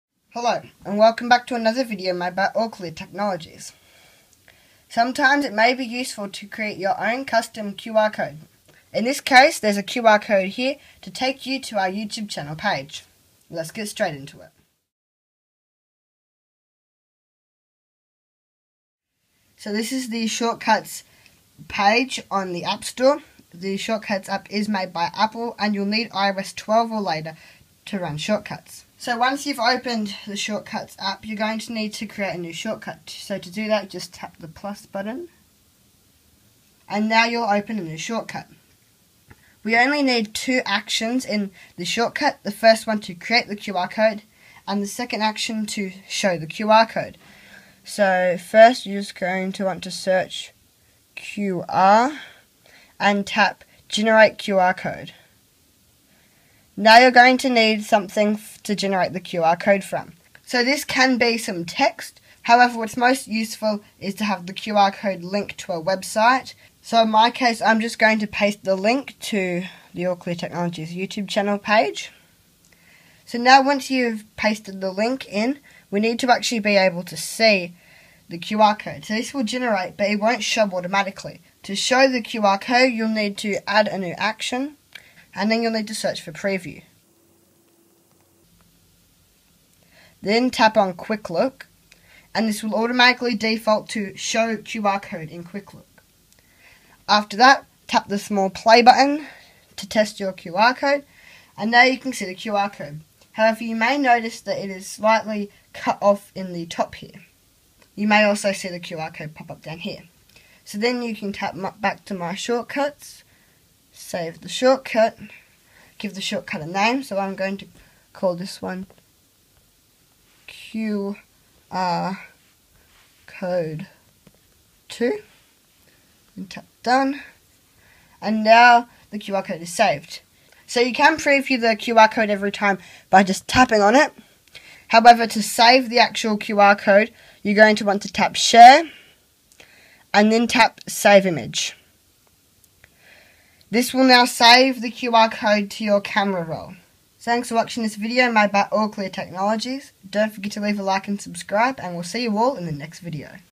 Create a QR Code using Shortcuts
In this video, we will be taking a look at how to create a QR code using Siri Shortcuts for iOS and iPad OS. There are a lot of QR code generators online, however, you may not know if they are linking the QR code to their website before redirecting to your site. This method uses the Siri Shortcuts app that is made by Apple.

You will need iOS or iPad OS 12 or higher to run Siri Shortcuts.